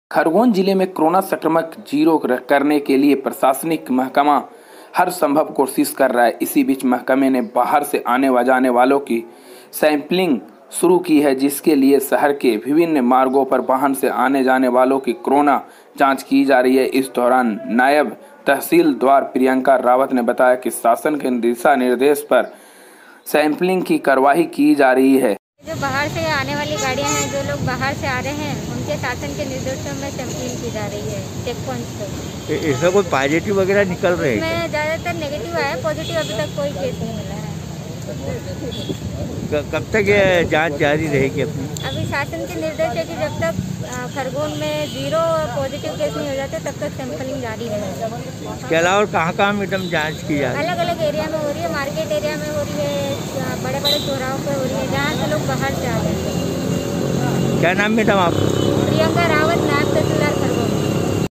कोरोना संक्रमण जिरो करने के लिये प्रशासनिक महकमा हर सम्भव कोशिश कर रहा है
खरगोन जिले में कोरोना संक्रमण जिरो करने के लिये प्रशासनिक महकमा हर सम्भव कोशिश कर रहा है इसी बिच महकमे ने बाहर से आनेजानेवालो की सैम्पलिंग शुरू की।
जिसके लिये शहर के विभिन्न मार्गो पर बाहर से आनेजानेवालो की कोरोना जांच की जा रही है।
इस दौरान नायब तहसीलदार प्रियंका रावत ने बताया की शासन के दिशानिर्देश पर सैम्पलिंग की कार्यवाही की जा रही है।
बाईट । प्रिंयंंका रावत
नायब तेहसीलदार